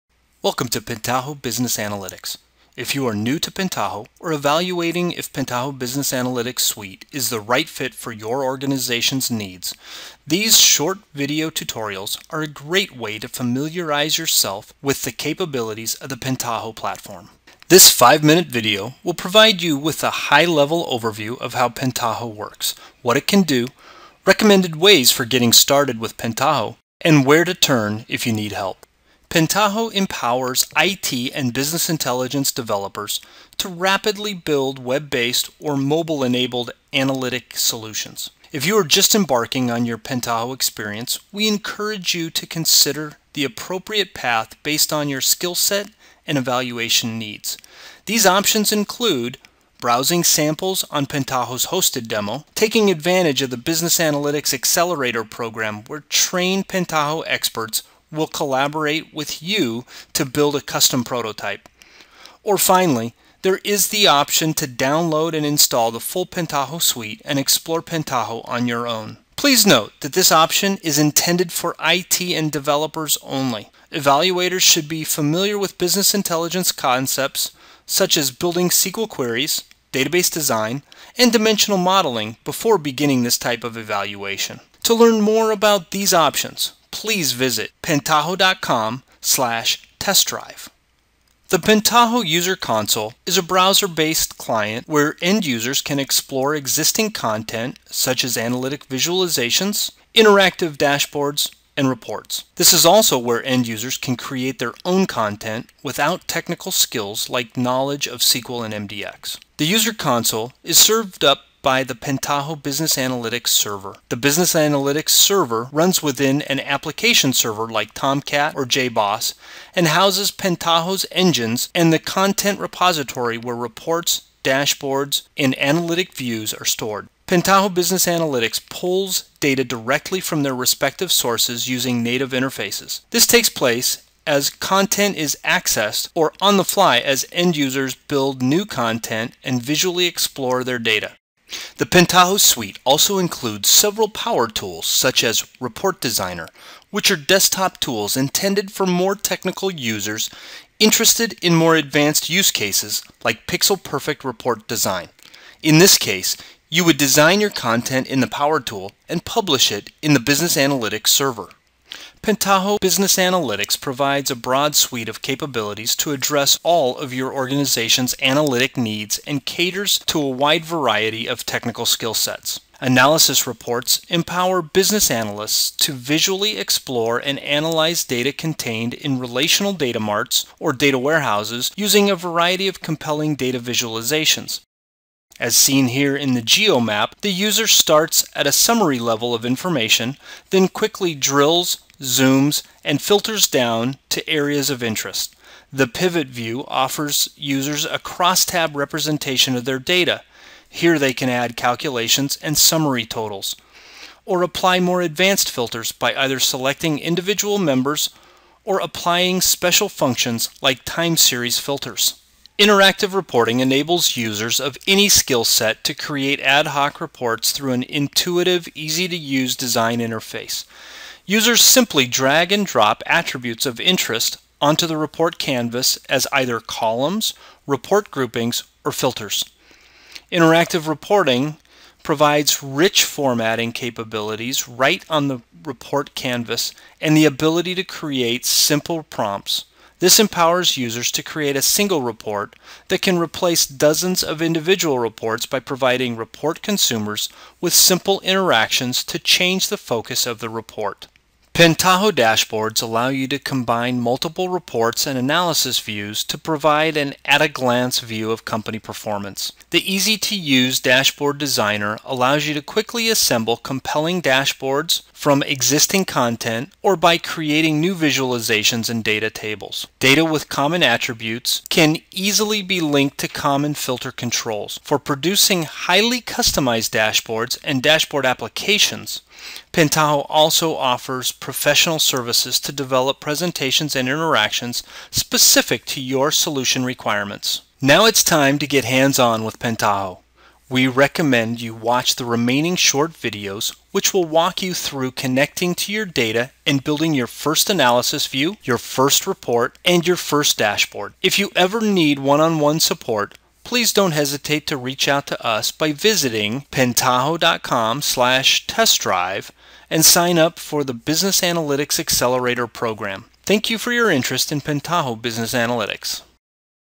Getting Started with Business Analytics
In this video, you'll get an overview of Pentaho's business analytics platform & a brief demo of visualization, reporting, and dashboards.